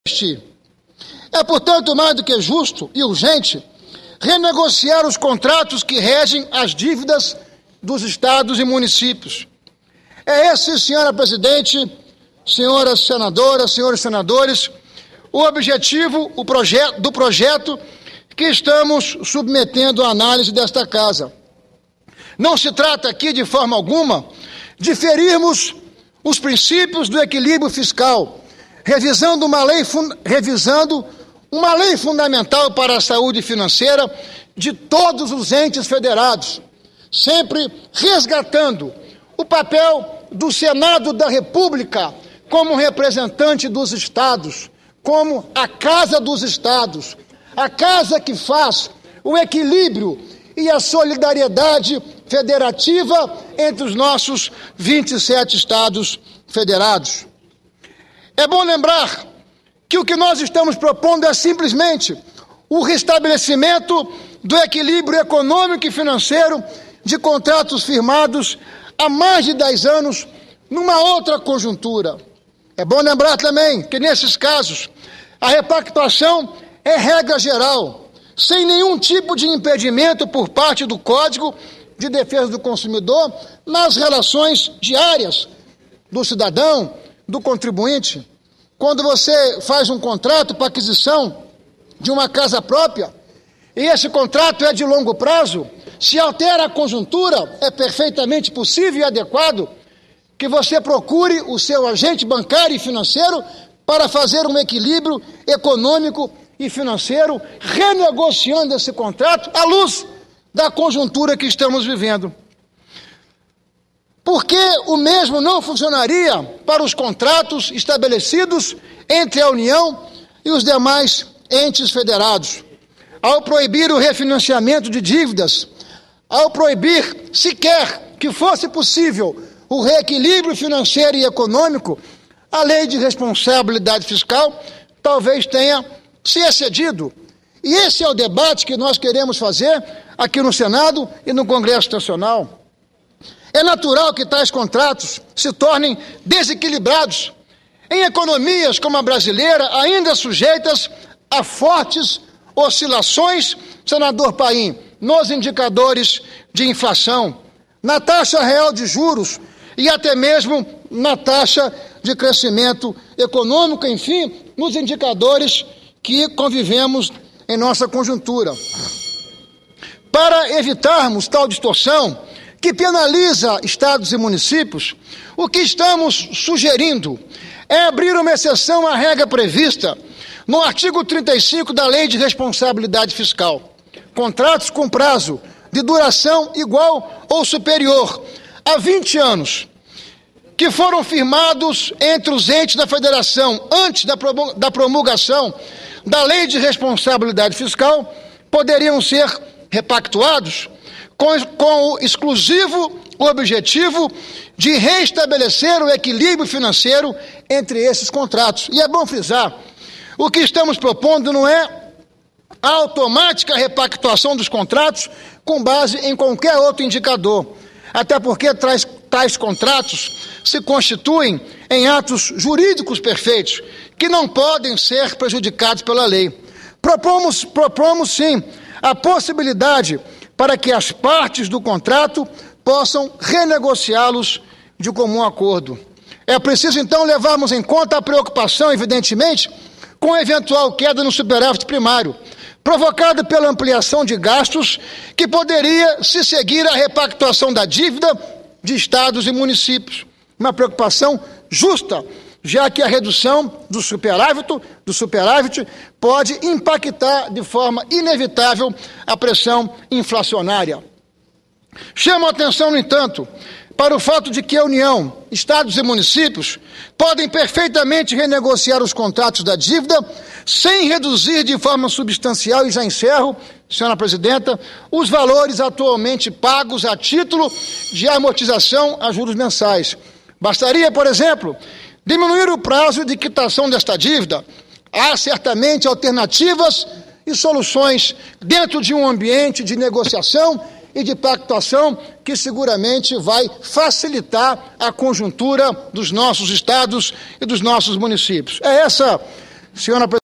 Ferraço anuncia Projeto sobre renegociação das dívidas estaduais e municipais
23/03/2011 - Plenário Discursos - Ferraço anuncia Projeto sobre renegociação das dívidas estaduais e municipais